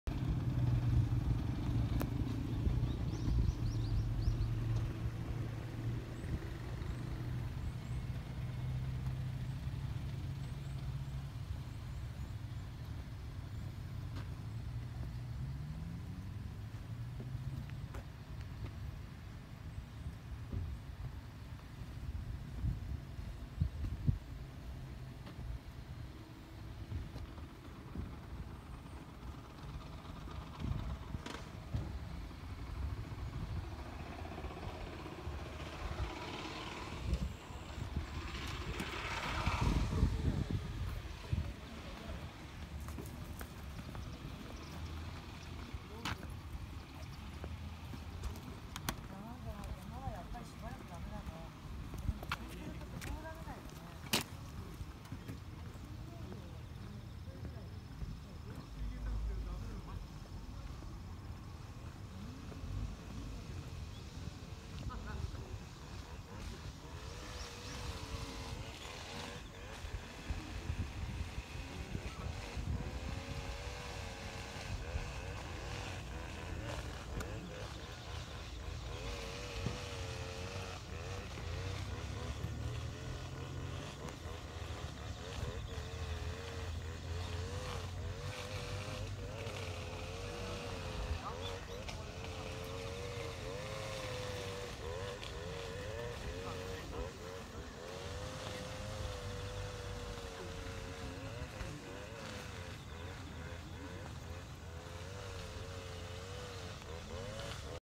エスティマで宮ヶ瀬にやってきました。
台風の影響で道には土砂流入後が散見され、粉塵が酷いです。また北岸ルートは通行止です。
圏央道は開通したものの、復旧工事で大渋滞してます。